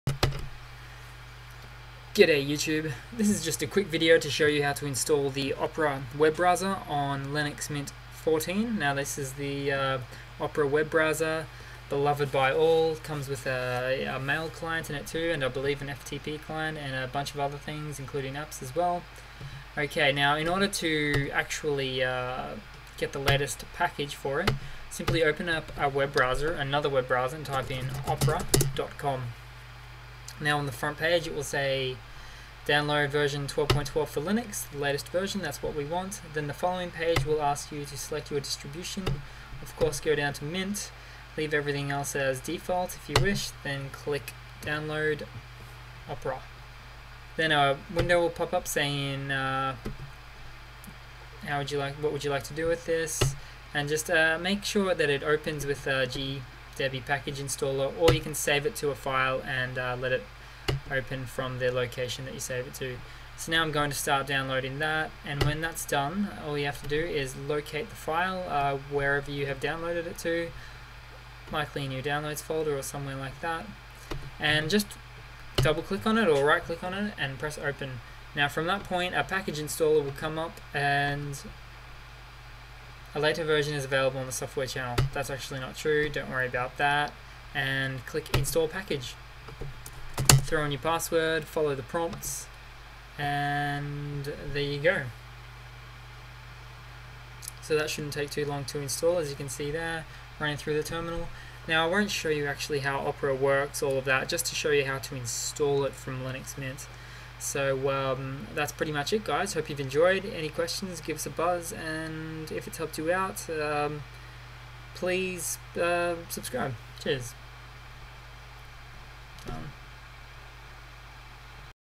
Install Opera Web Browser in Linux Mint 14
the beloved opera web browser - how to install on linux mint 14!